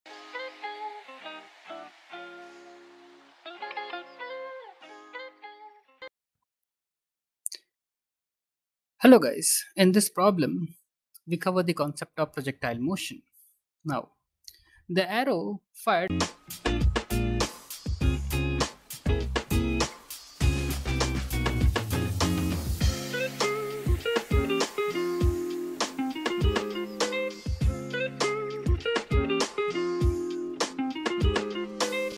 Question 18 (12 points) An archer fires an arrow horizontally towards a target that is some distanc…
Question 18 (12 points) An archer fires an arrow horizontally towards a target that is some distance away. The archer aims directly at the center of the target. If the arrow leaves the bow with a speed of 247 m/s and hits the target 10 cm below center, how far away (in meters) is the target? Your Answer: Answer